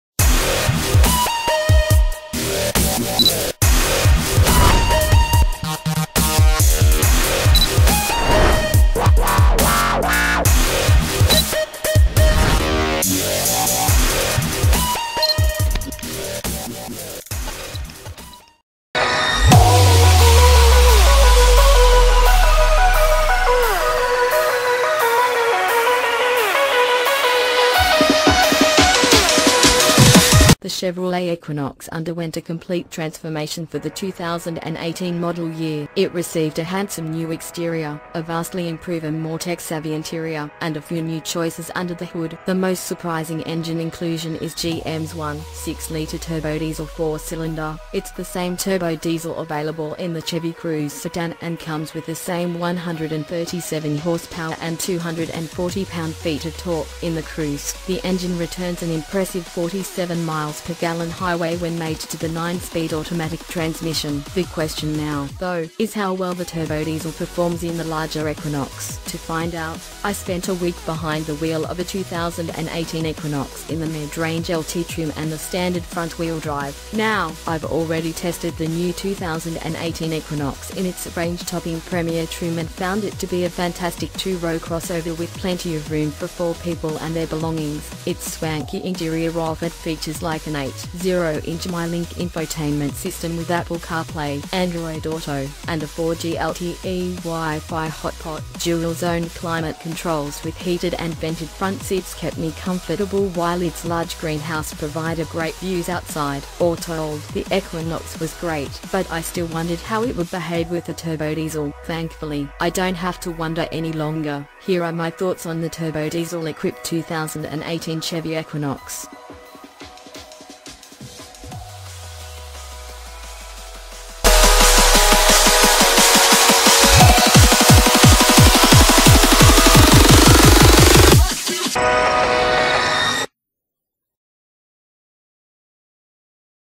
HOT NEWS!!! 2018 Chevrolet Equinox Turbodiesel Overview
HOT NEWS!!! 2018 Chevrolet Equinox Turbodiesel Overview - The Chevrolet Equinox underwent a complete transformation for the 2018 model year. It received a handsome new exterior, a vastly improve and more tech-savvy interior, and a few new choices under the hood. The most surprising engine inclusion is GM’s 1.6-liter turbodiesel four-cylinder. It’s the same turbodiesel available in the Chevy Cruze sedan and comes with the same 137 horsepower and 240 pound-feet of torque. In the Cruze, the engine returns an impressive 47 mpg highway when mated to the nine-speed automatic transmission. The question now, though, is how well the turbodiesel performs in the larger Equinox.

Popular Post !